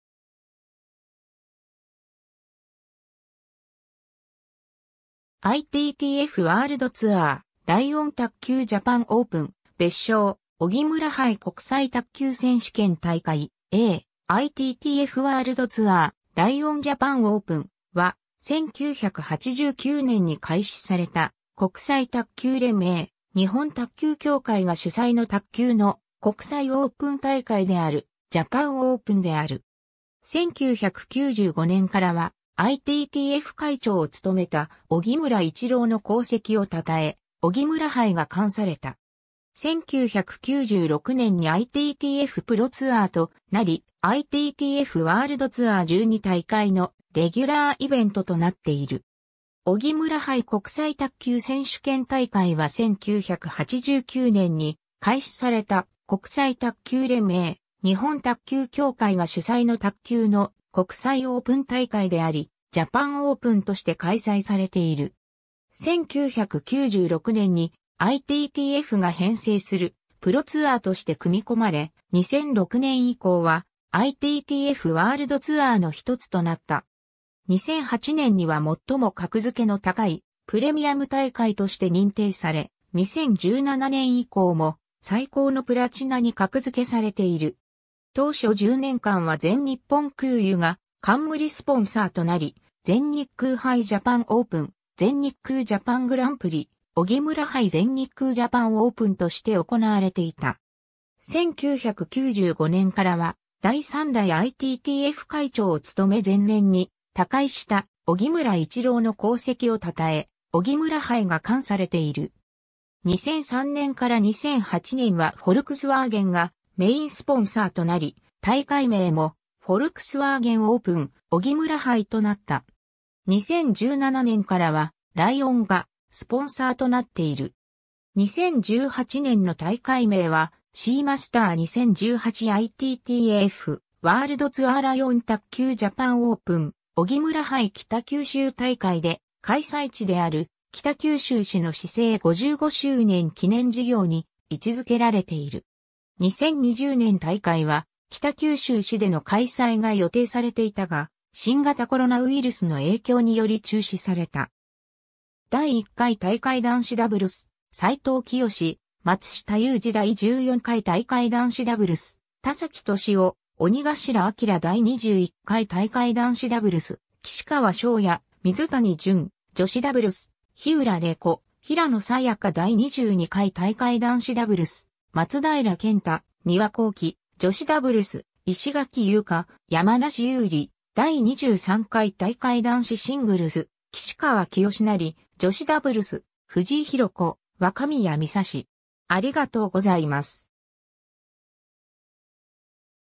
荻村杯国際卓球選手権大会
ITTFワールドツアー, ライオン卓球ジャパンオープン〔別称：荻村杯国際卓球選手権大会(おぎむらはいこくさいたっきゅうせんしゅけんたいかい)、英: ITTF World Tour, LION Japan Open〕は、1989年に開始された、国際卓球連盟（ITTF）、日本卓球協会が主催の卓球の国際オープン大会である、ジャパンオープンである。
1995年からは、ITTF会長を務めた荻村伊智朗の功績を称え「荻村杯」が冠された。
1996年にITTFプロツアーとなり、ITTFワールドツアー12大会のレギュラーイベントとなっている。
 荻村杯国際卓球選手権大会は1989年に開始された、国際卓球連盟（ITTF）、日本卓球協会が主催の卓球の国際オープン大会であり、ジャパンオープンとして開催されている。
1996年にITTFが編成するプロツアーとして組み込まれ、2006年以降はITTFワールドツアーの1つとなった。
2008年には最も格付けの高いプレミアム大会として認定され、2017年以降も最高のプラチナに格付けされている。
当初10年間は全日本空輸が冠スポンサーとなり、「全日空杯ジャパンオープン」（1989年 - 1991年）、「全日空ジャパングランプリ」（1993年 - 1997年）、「荻村杯全日空ジャパンオープン」（1998年）として行われていた。
1995年からは、第3代ITTF会長を務め前年に他界した荻村伊智朗の功績を称え「荻村杯」が冠されている。
2003年から2008年はフォルクスワーゲンがメインスポンサーとなり、大会名も「フォルクスワーゲンオープン荻村杯」となった。
2017年からは「ライオン」がスポンサーとなっている。
 2018年の大会名は「Seamaster 2018 ITTF ワールドツアーライオン卓球ジャパンオープン荻村杯北九州大会」で、開催地である北九州市の市制55周年記念事業に位置付けられている。
 2020年大会は北九州市での開催が予定されていたが、新型コロナウイルスの影響により中止された。
 第1回大会（1989年） 男子ダブルス：斎藤清・松下雄二 第14回大会（2002年）男子ダブルス：田崎俊雄・鬼頭明 第21回大会（2009年）男子ダブルス：岸川聖也・水谷隼、女子ダブルス：樋浦令子・平野早矢香 第22回大会（2010年）男子ダブルス：松平健太・丹羽孝希、女子ダブルス：石垣優香・山梨有理 第23回大会（2011年）男子シングルス：岸川聖也、女子ダブルス：藤井寛子・若宮三紗子